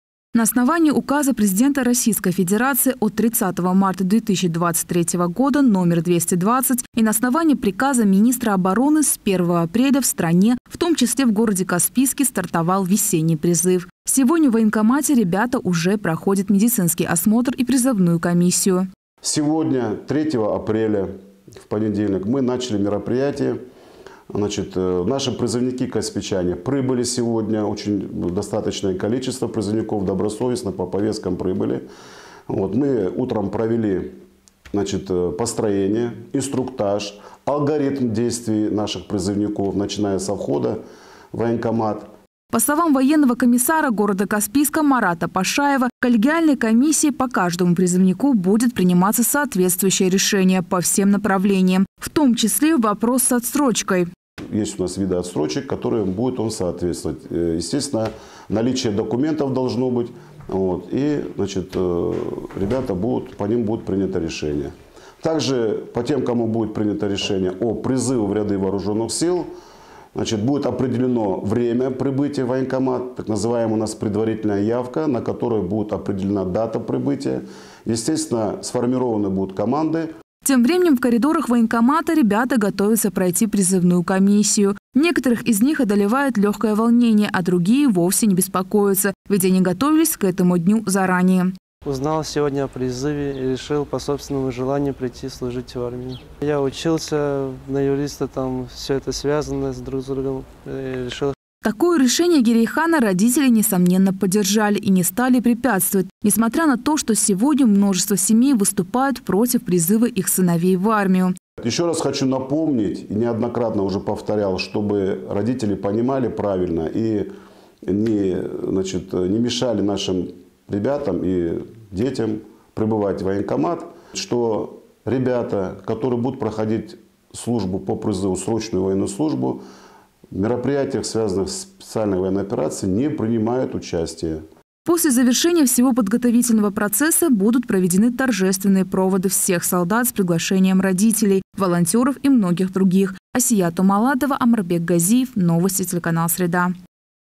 Весенний призыв стартовал в Каспийске
На основании Указа Президента РФ от 30 марта 2023 года №220 и на основании приказа Министра Обороны с 1 апреля в стране, в том числе в Каспийске, стартовал, весенний призыв. Сегодня в военкомате ребята уже проходят медицинский осмотр и призывную комиссию.